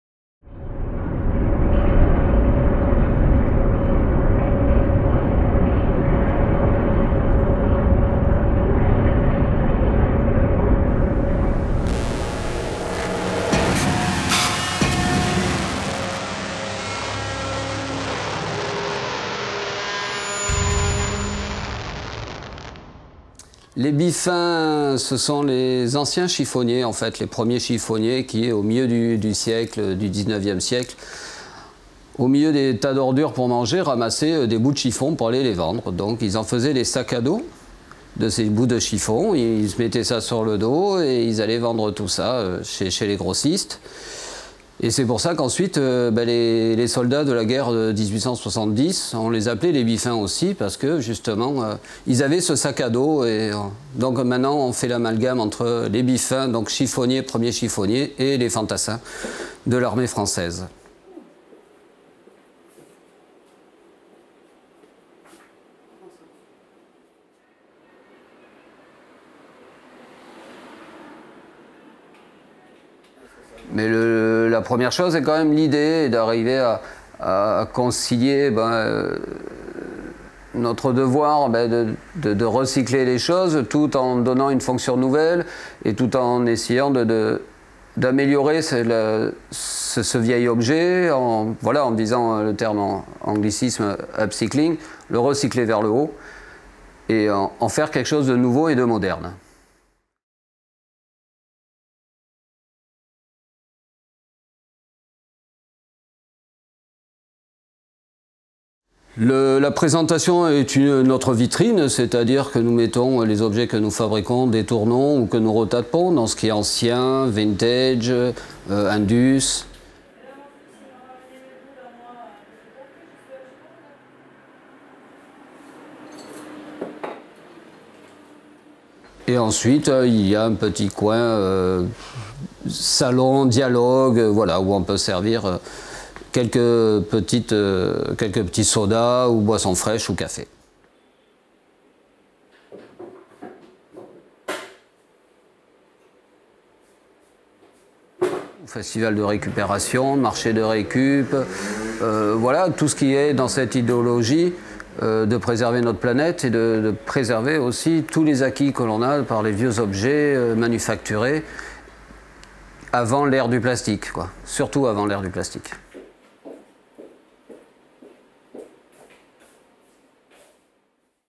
LES BIFFINS OCCITANS
Septembre 2017-Villemurs-sur-Tarn (31)
Pilote d'émission webtv sur les savoir-faire des artisans/commerçants du Canton de Villemur-sur-Tarn.